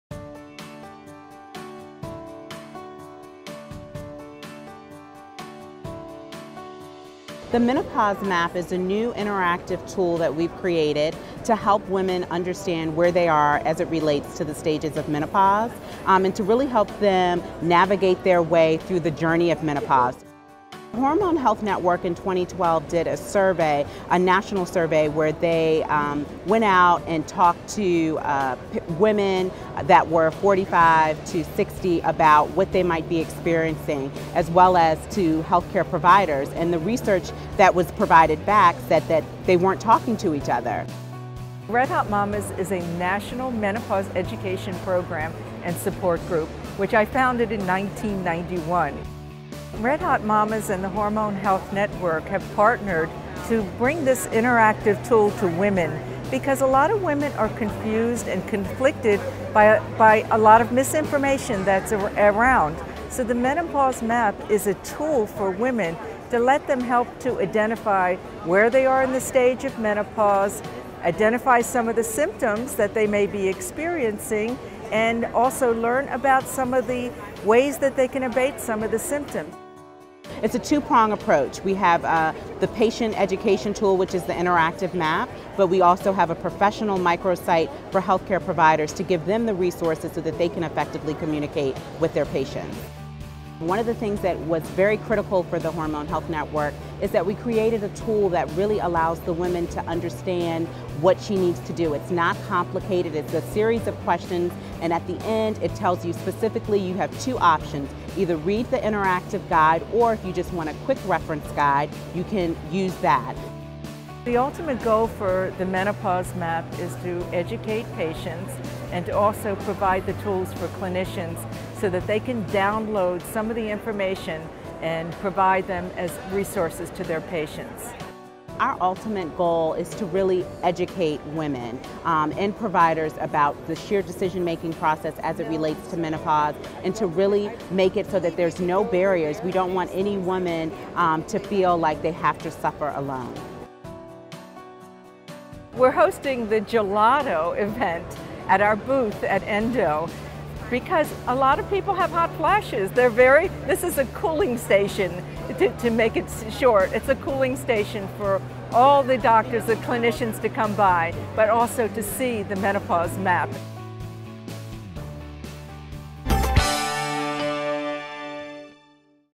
Menopause Map - Interactive Online Tool
Surveys show that women don't talk to their doctors about perimenopausal symptoms either out of embarrassment or lack of knowledge. A new interactive tool has launched, the Menopause Map, aimed at teaching women about their symptoms and educating them about how to communicate to their doctors for diagnosis and managing those symptoms. The Menopause Map is a joint effort of the Hormone Health Network and the Red Hot Mamas. Watch to learn more about this important new online tool for physicians and patients.